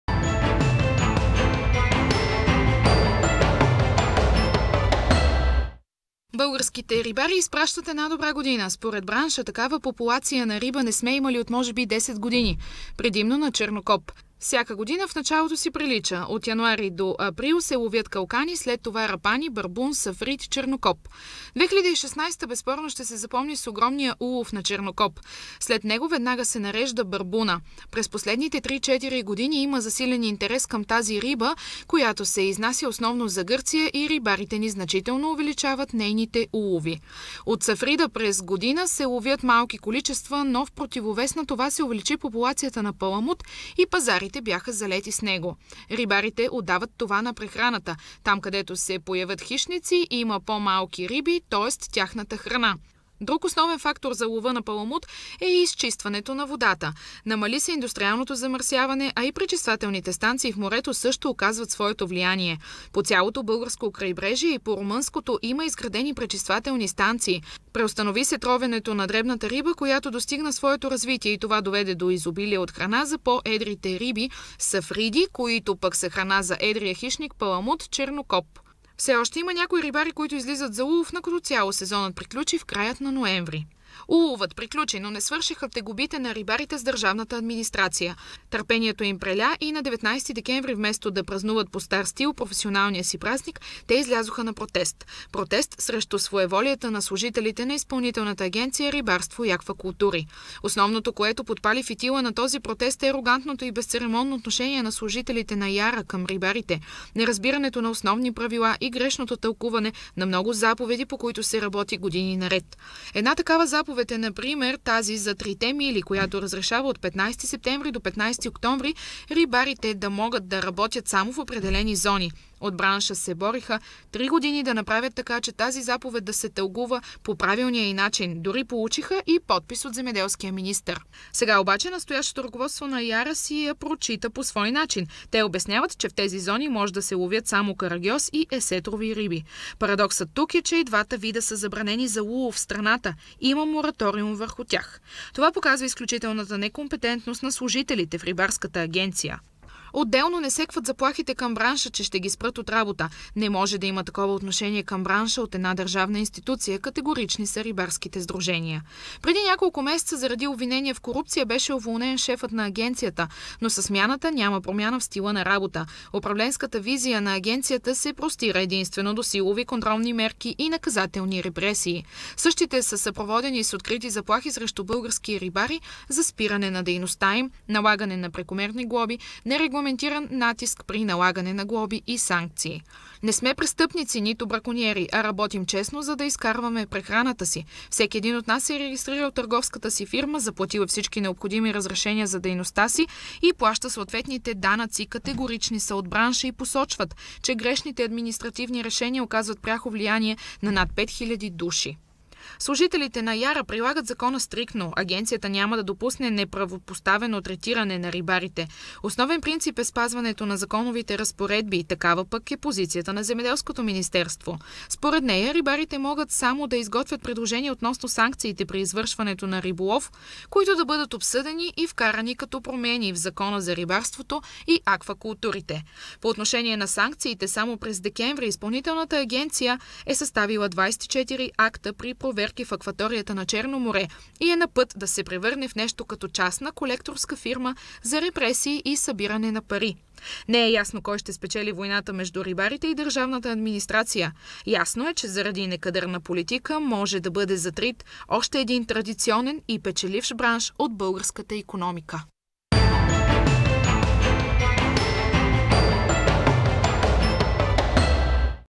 Кой ще спечели рибната война
Българските рибари изпращат една добра година